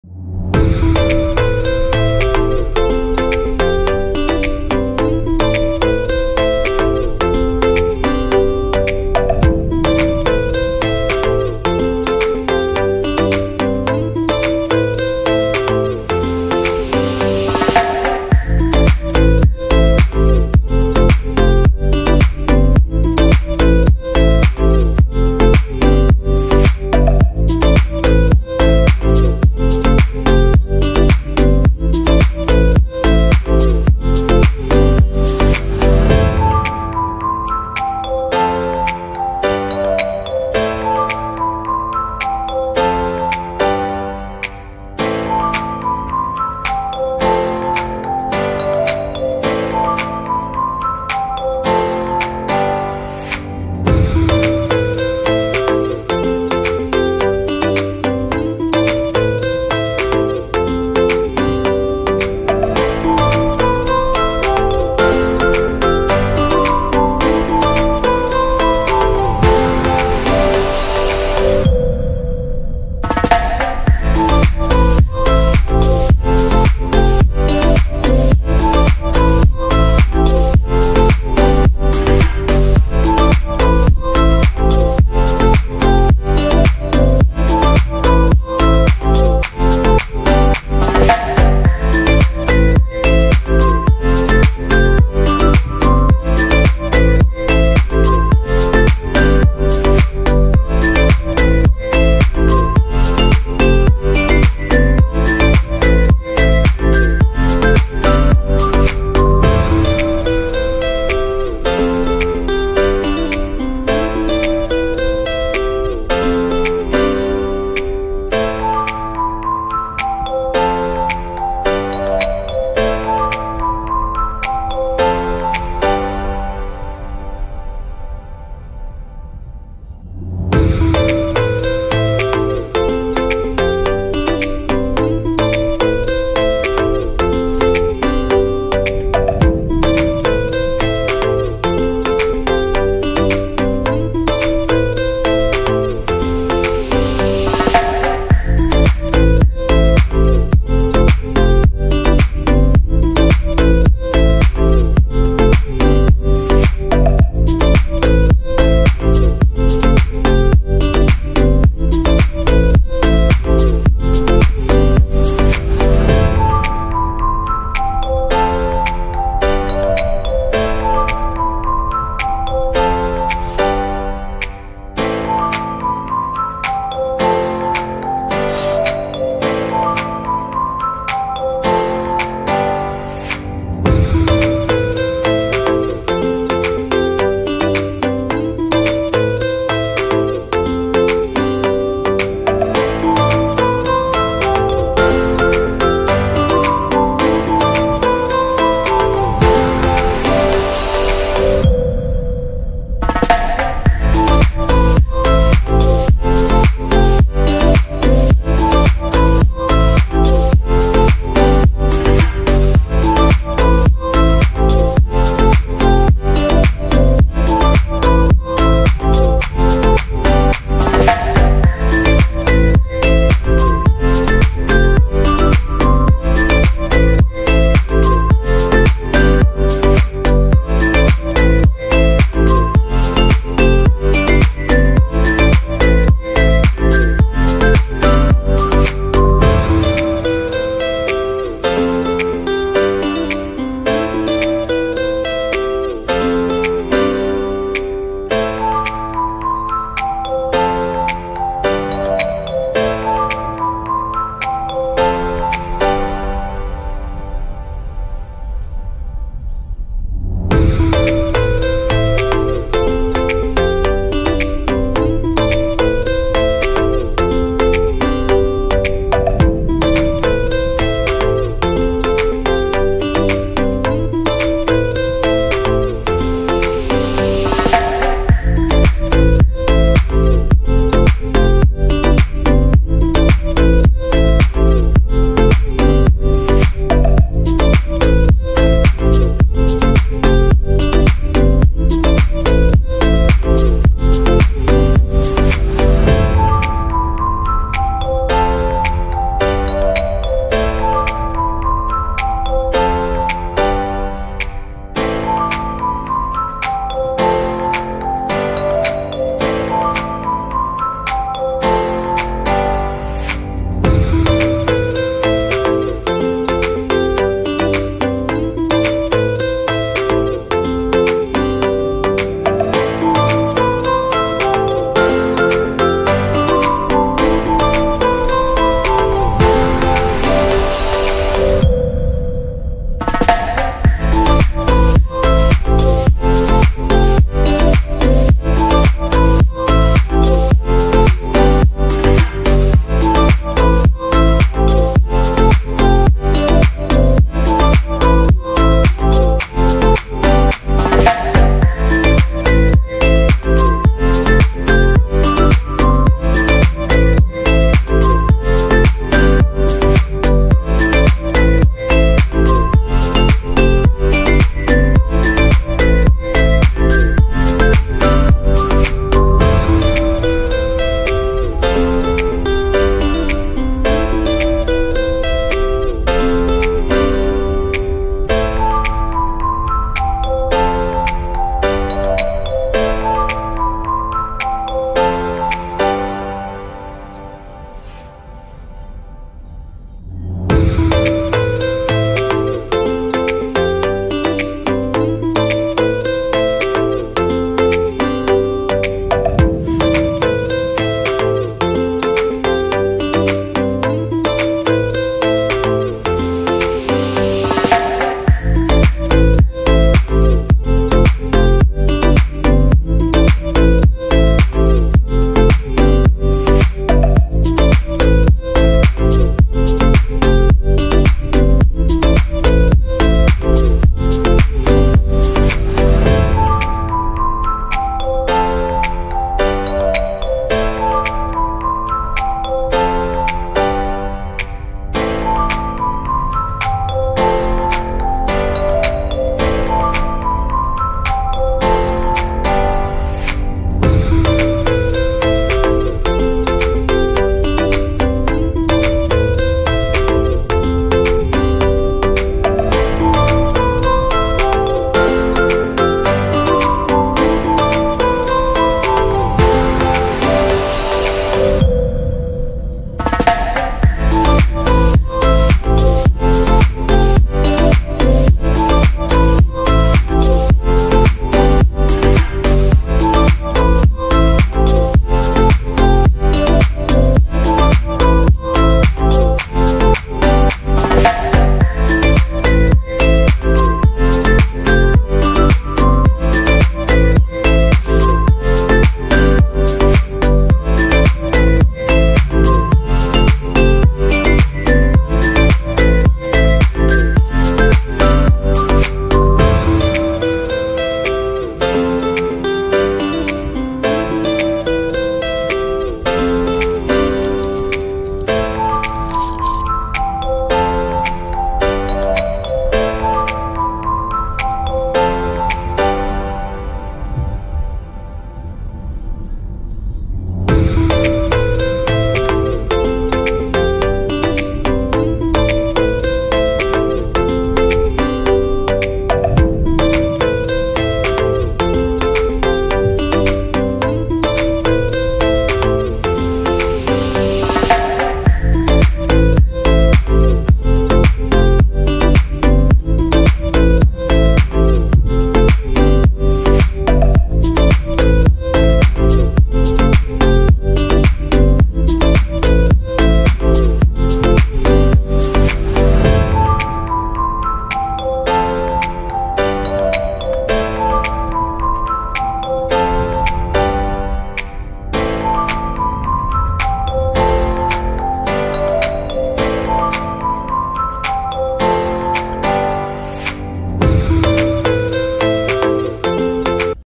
Um passeio pelo Cais do Sodré e Praça do Comércio com a Favaron Viagens
Um pouco da beleza das ruas de Lisboa, passando pelo Cais do Sodré e Praça do Comércio. Final de tarde, com este por do sol lindo.